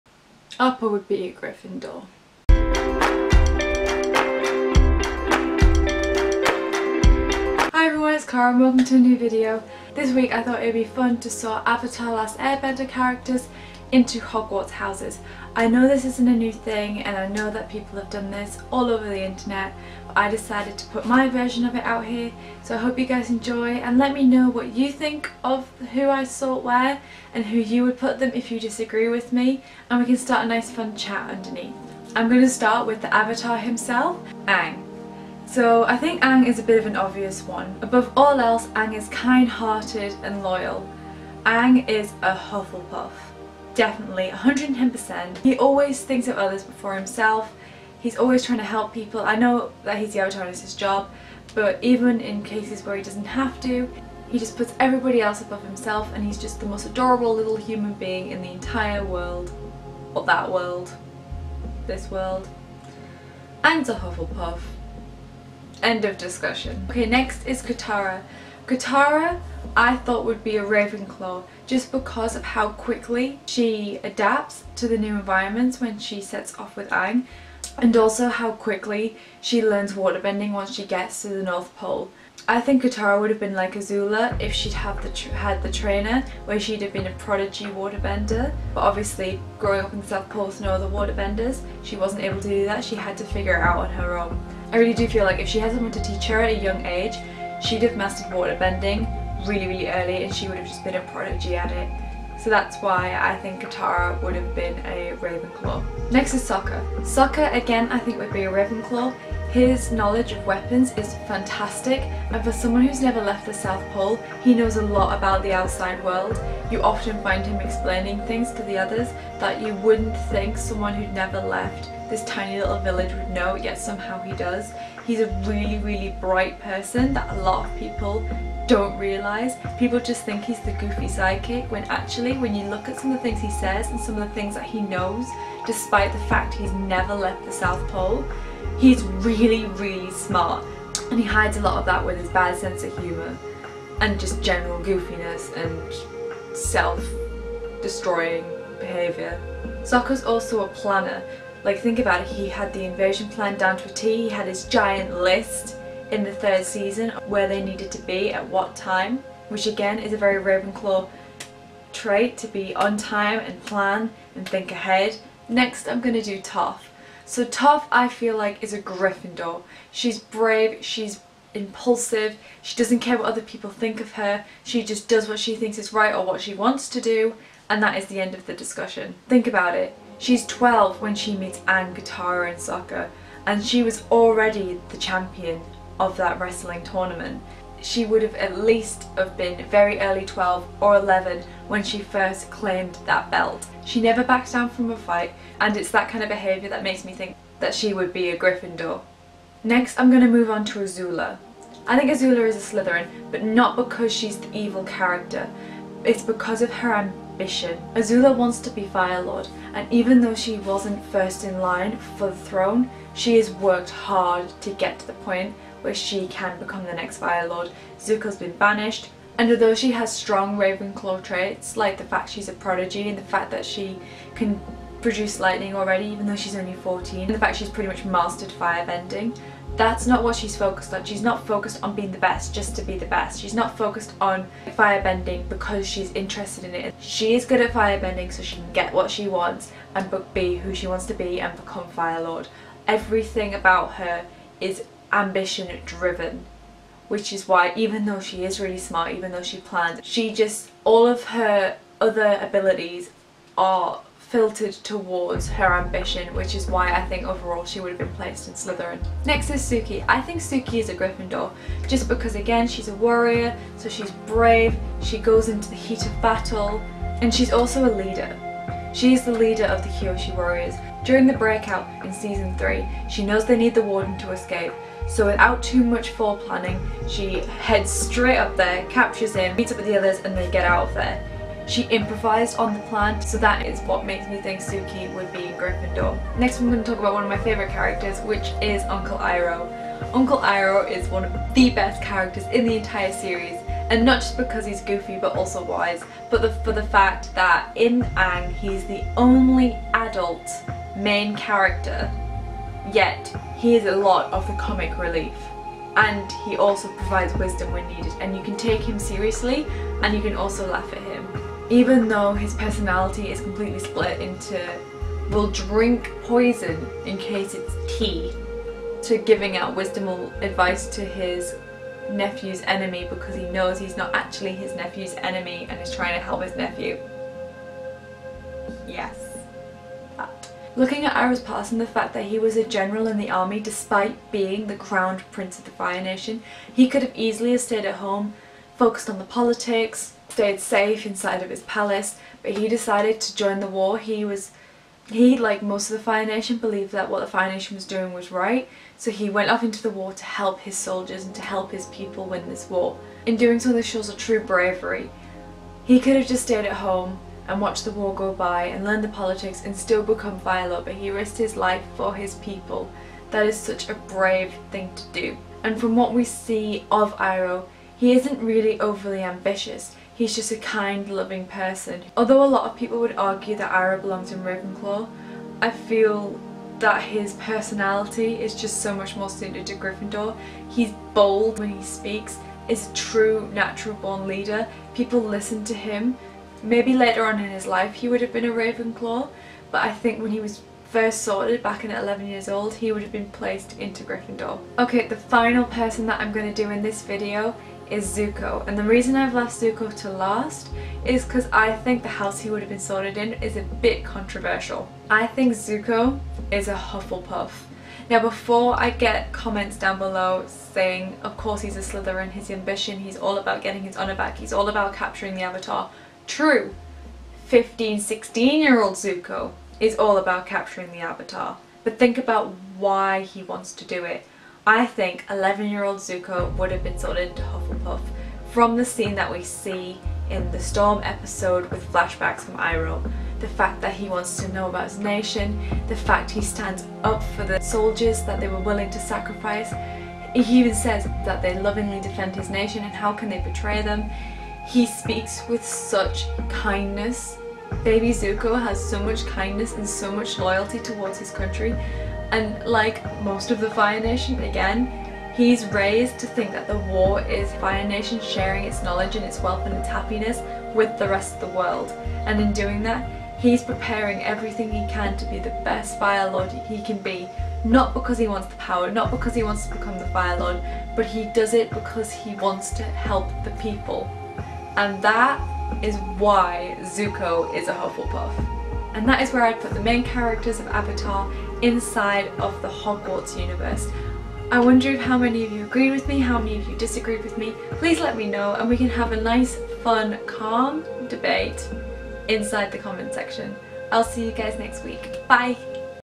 Sort Avatar Characters into Houses
A fun video that puts the characters of Avatar into Hogwarts and sorts them. What houses do you guys think they belong? Do you agree with me? I think there is one character who will shock you :) xx